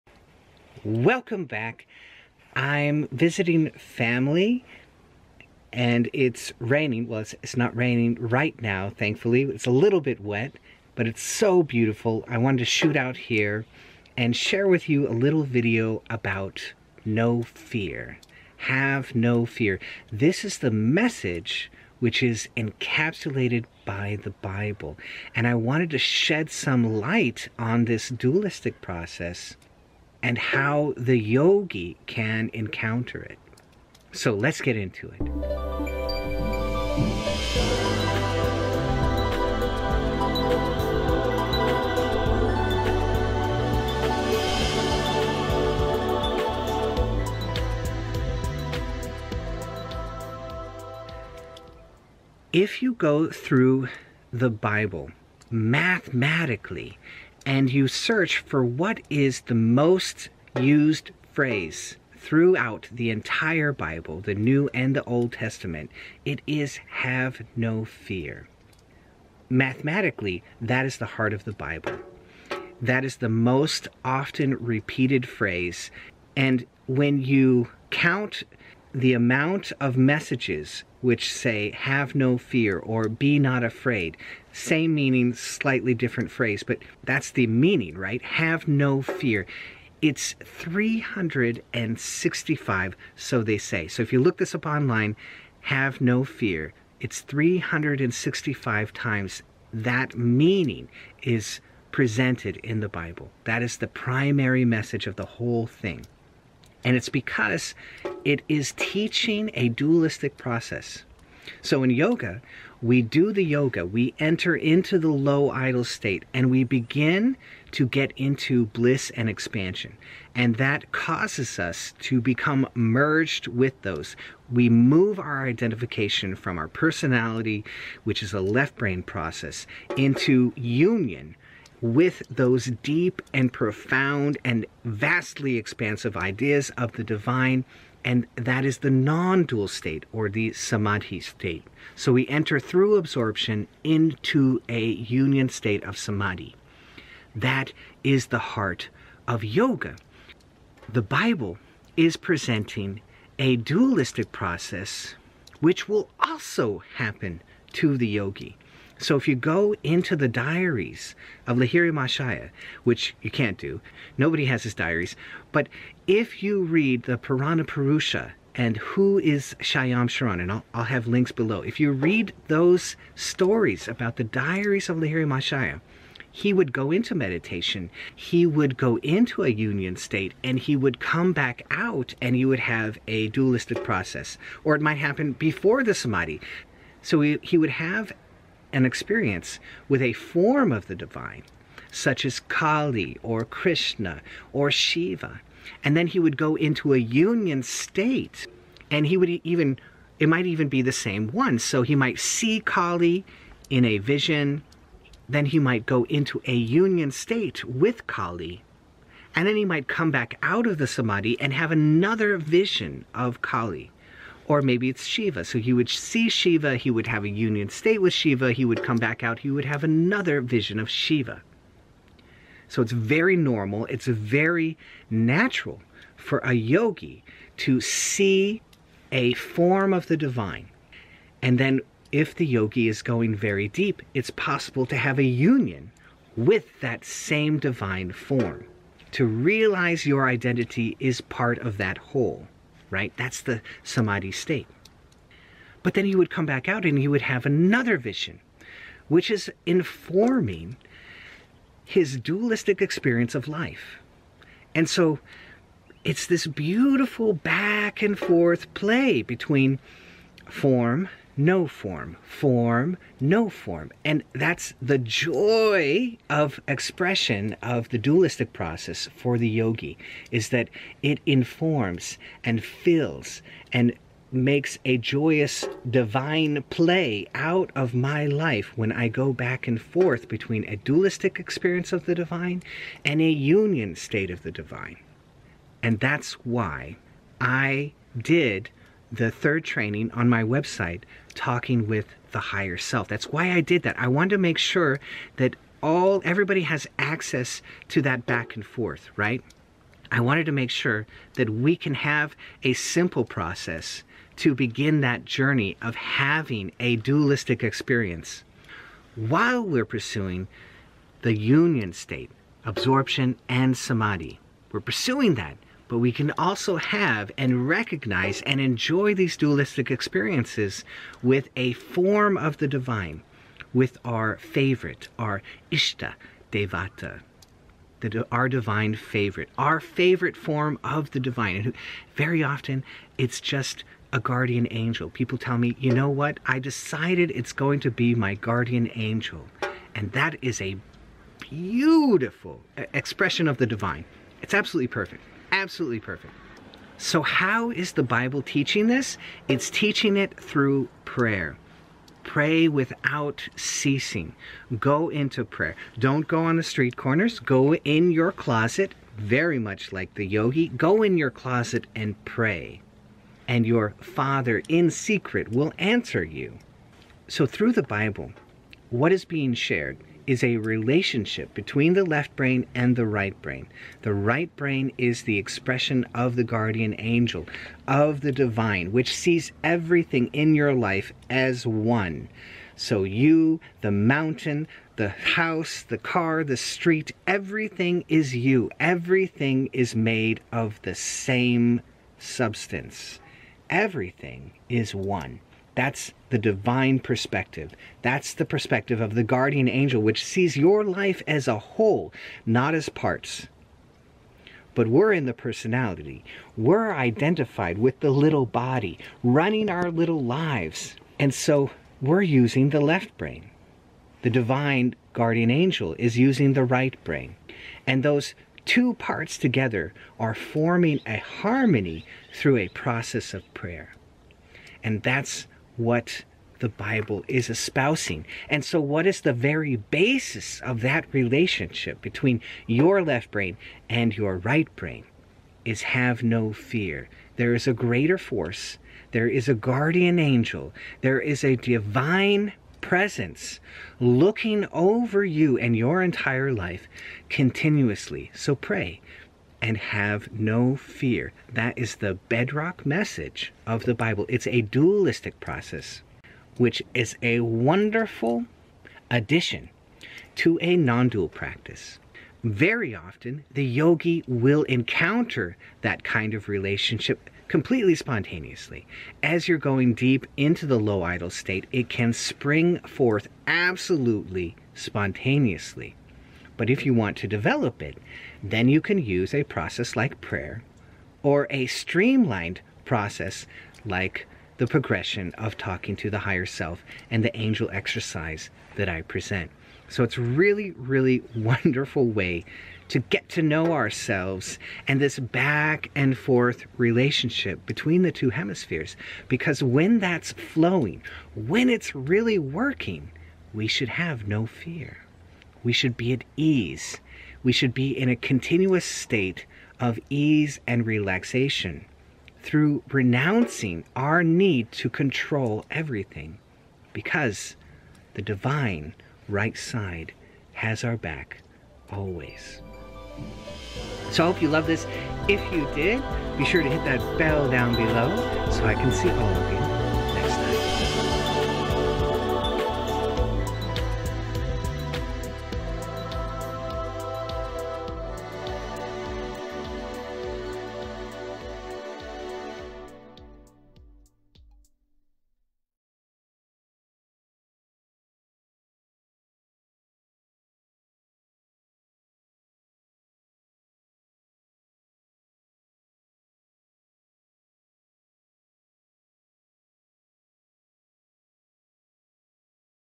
Fear Not and a Dualistic Process - Yogi Explains
Mathematically, the core message of the Bible is "Fear not," which is the heart of a Dualistic Process of leaning on the Divine right side of your brain.

Purana Purusha
Who Is Shama Churn

Thank you to Doug Gemmell for the amazing music!